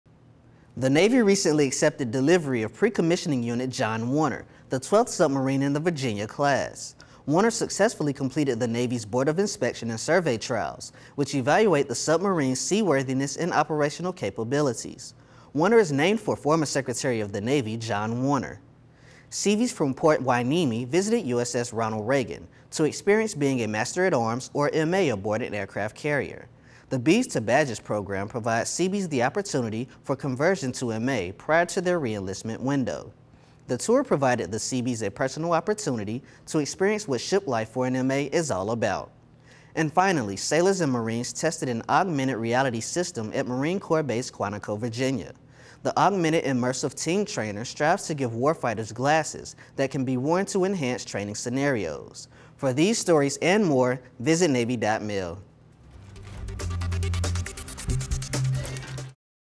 Headlines for Monday, June 29, 2015 (HL 29)
Navy accepts delivery of PCU John Warner. Seabees experience being a master-at-arms at sea. Sailors and Marines test augmented reality glasses.